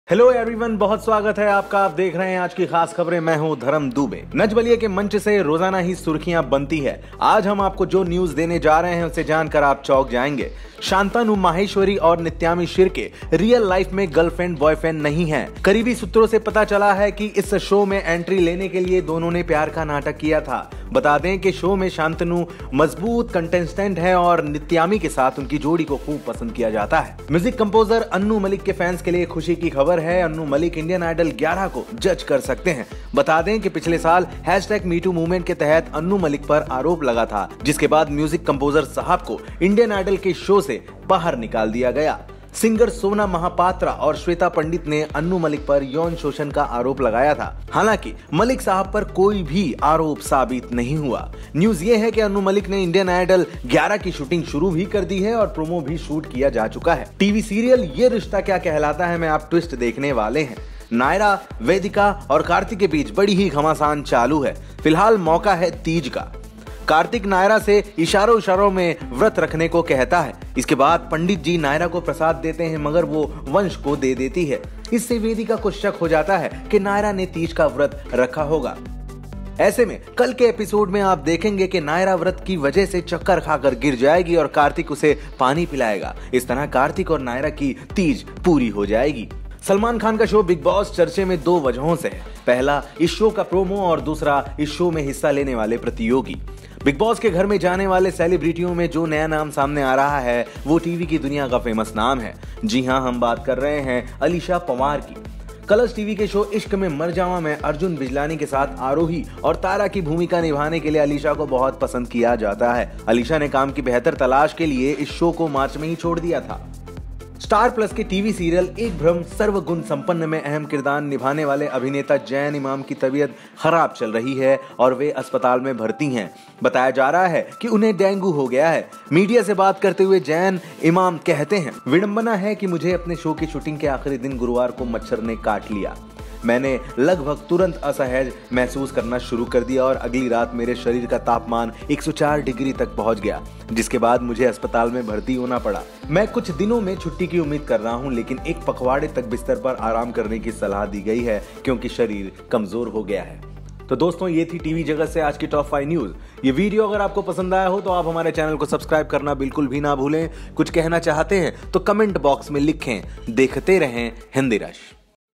OMG: Shantanu Maheshwari और Nityaami Shirke रियल लाइफ में नहीं हैं कपल, टीवी एक्टर को हुआ डेंग्यू
Aaj Ki Khas Khabren में आप देख रहे हैं Shantanu Maheshwari और Nityaami Shirke रियल लाइफ में नहीं हैं कपल, इंडियन आइडियल (Indian Idol) को जज करेंगे अनु मलिक (Anu Malik), कार्तिक के लिए नायरा ने रखा तीज का व्रत, टीवी की ये अभिनेत्री जाएगी बिग बॉस (Bigg Boss) में, टीवी के अभिनेता को हुआ डेंग्यू, अस्पताल में भर्ती